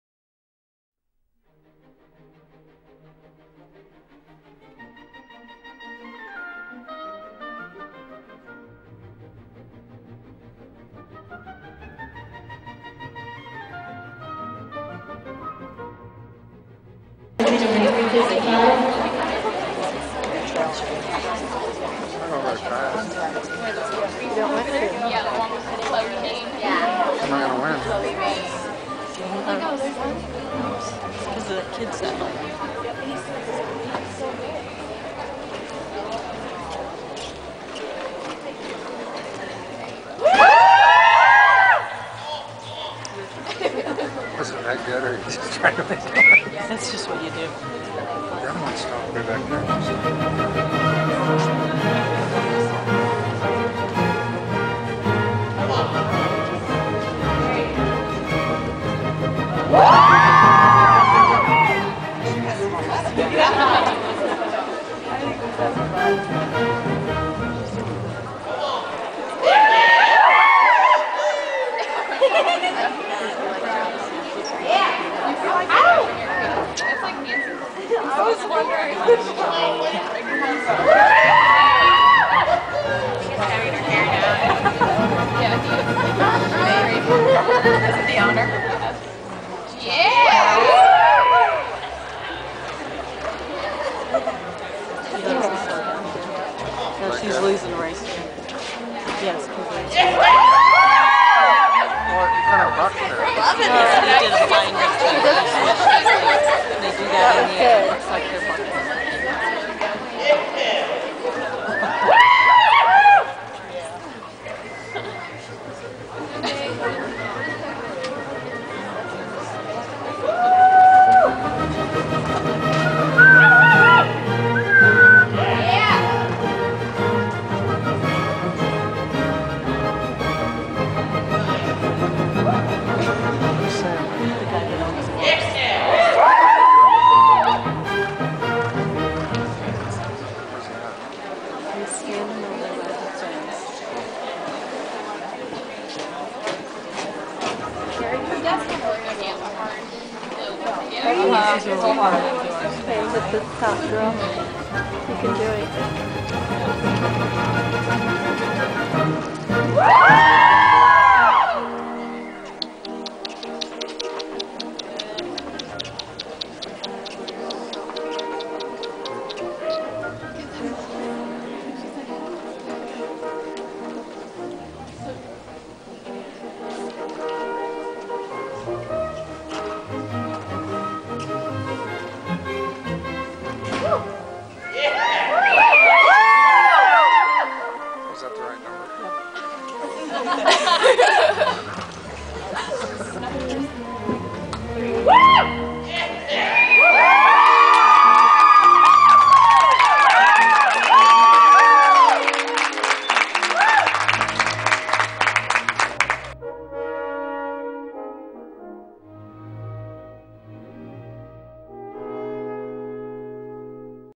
Kelsey Reining Seminationals_0001.wmv
Kelsey competing in Open Reining at the IHSA Seminationals show.